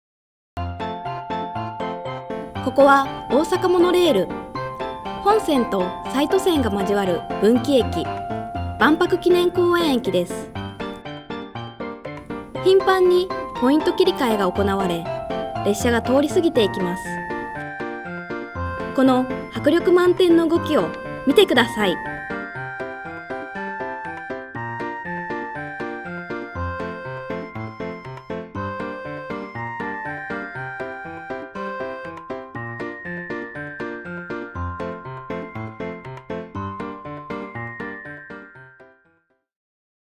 線路が曲がる？！万博記念公園駅の珍景【公式】
万博記念公園駅には、モノレールでは珍しい分岐器があります！
動画は10倍速ですが、こうやって見ると、なんだか生きものみたい……？